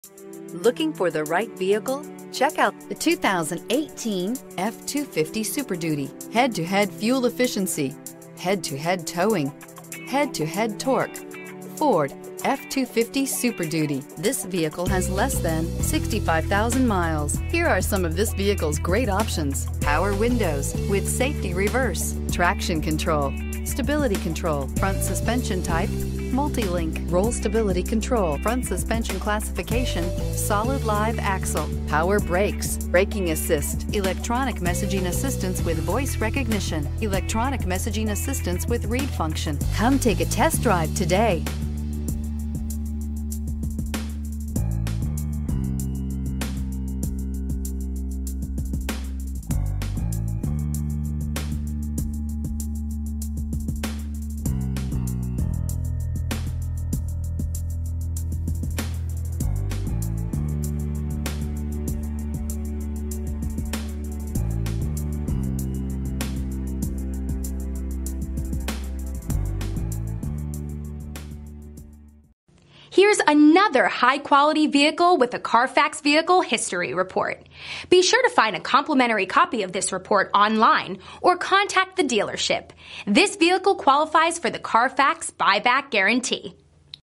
2018 Ford F-250 Super Duty Houston TX NC11009
2018 Ford F-250 Super Duty XLT

This Shadow Black 2018 Ford F-250 Super Duty XLT might be just the crew cab 4x4 for you.  It comes with a 8 Cylinder engine.  We're offering a great deal on this one at $59,875.  For a good-looking vehicle from the inside out, this crew cab 4x4 features a beautiful shadow black exterior along with a medium earth gray interior.  Call or visit to talk with one of our vehicle specialists today!